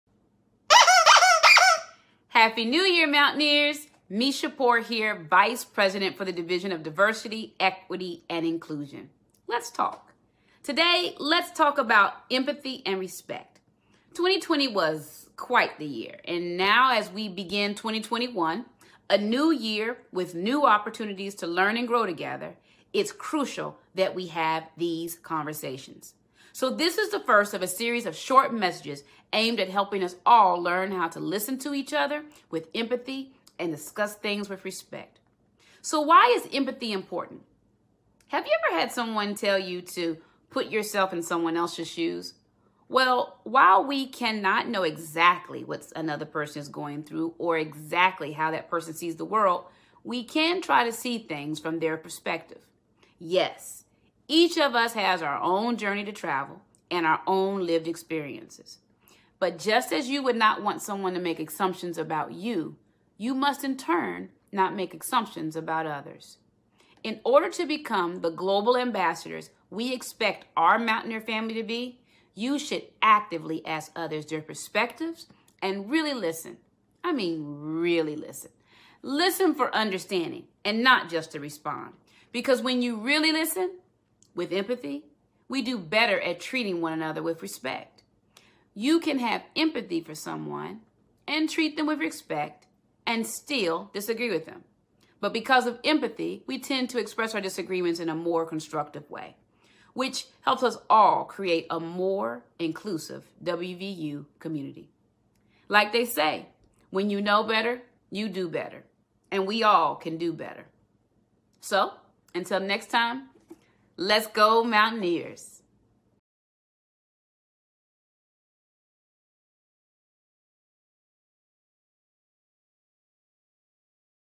Let's Talk: Empathy and Respect
Meshea L. Poore, Esq., Vice President for the Division of Diversity, Equity and Inclusion, discusses the importance of empathy and respect.